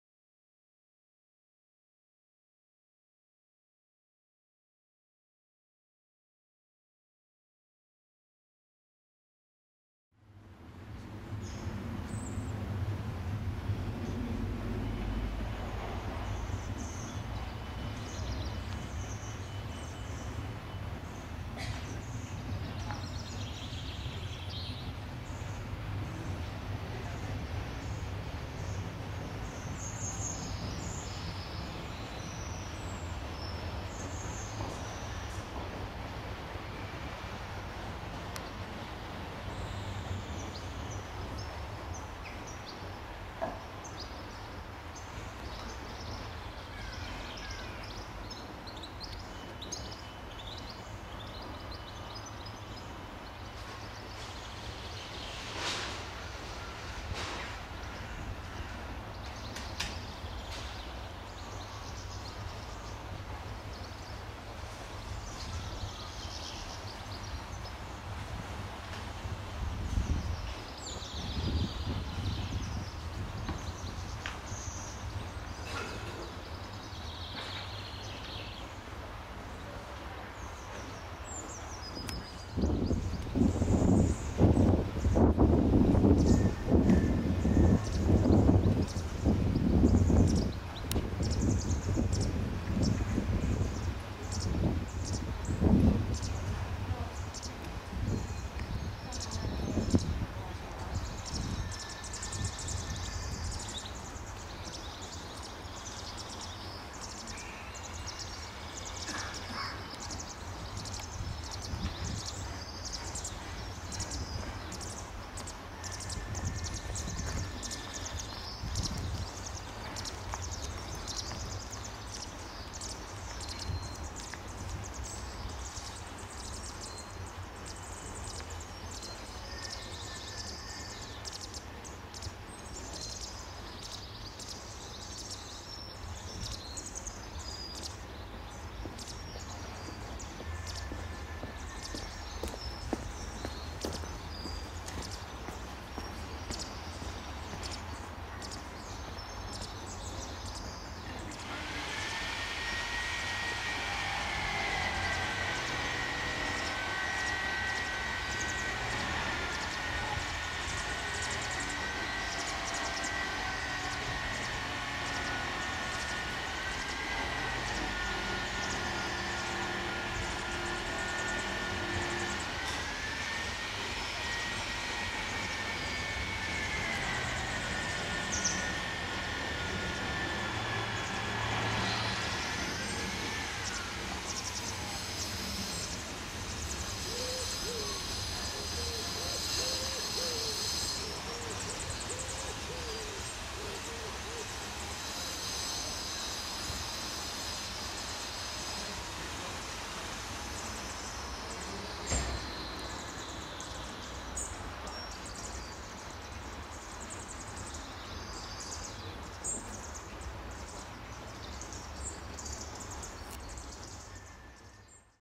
Pied Wagtails at Hitchin Town Centre - June 2014
Wondering around the graveyard of St Mary's Church in Hitchin Town Centre some Pied Wagtails were busy feeding their young amongst the gravestones, they were happy as wagtails are for you to get up closer to them too :)